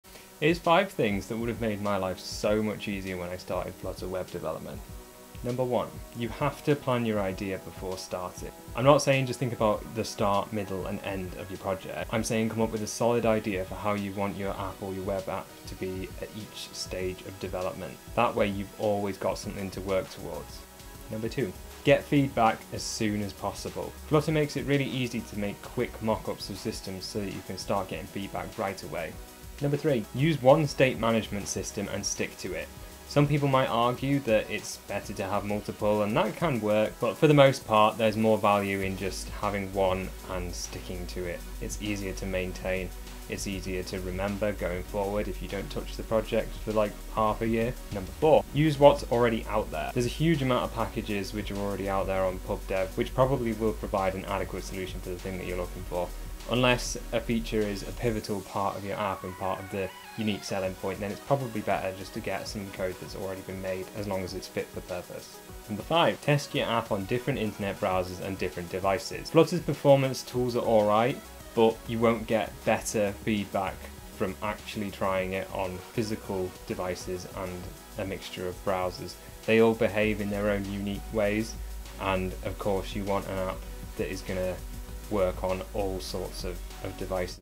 Things to know BEFORE starting your next Flutter project
In this video I give a list of useful things to know before starting a Flutter project. These will make development more streamlined and will cause less hurdles along the way. In my experience, one of the most time-consuming aspects of software development is needing to backtrack because of a decision that was made early-on, usually through lack of experience in that area.